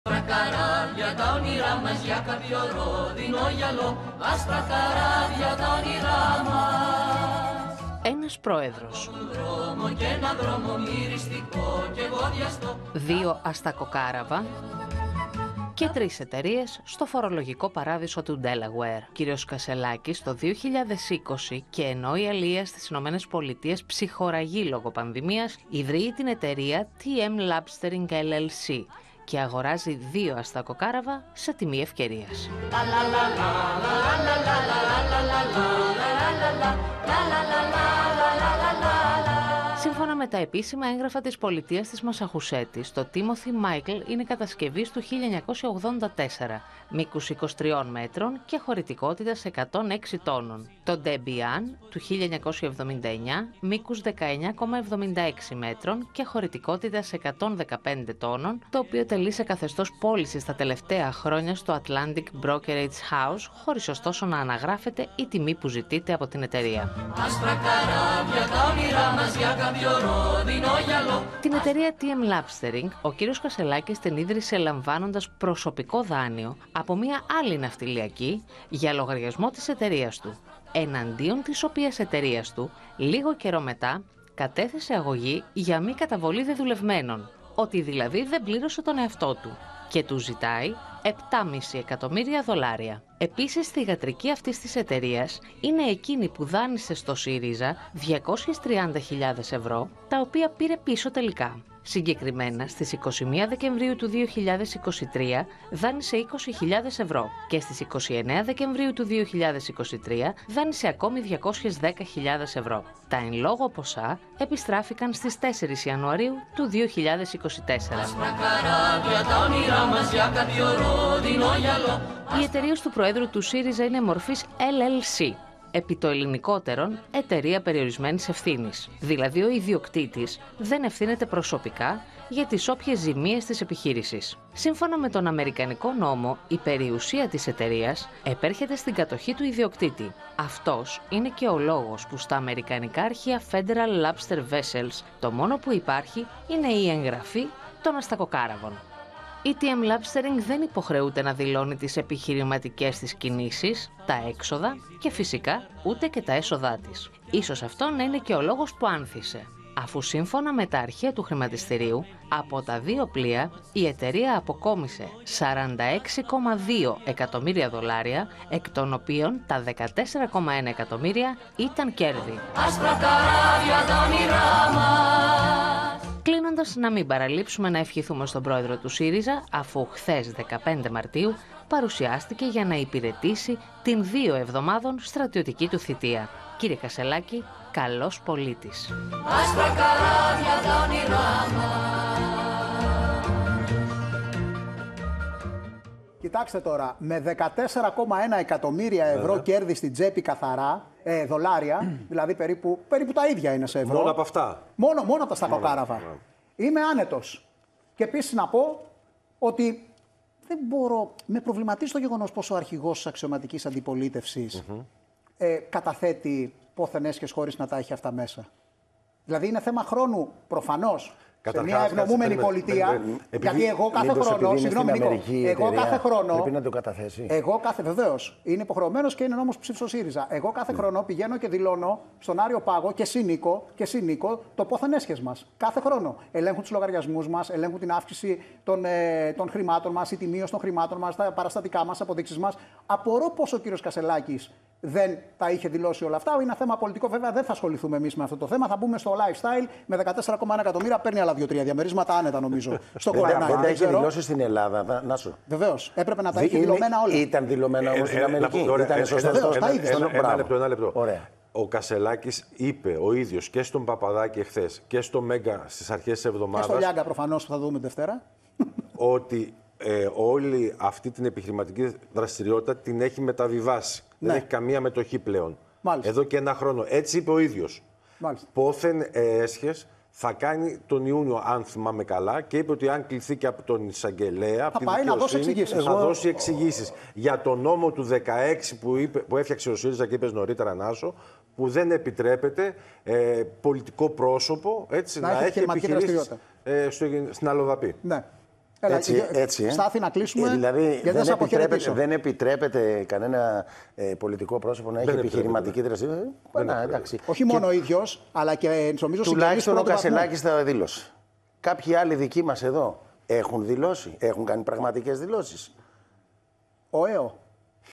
Τα αστακοκάραβα του Στέφανου Κασσελάκη | OPEN TV
Οι εταιρείες, τα δάνεια και οι ψαριές του αρχηγού της αξιωματικής αντιπολίτευσης, Στέφανου Κασσελάκη.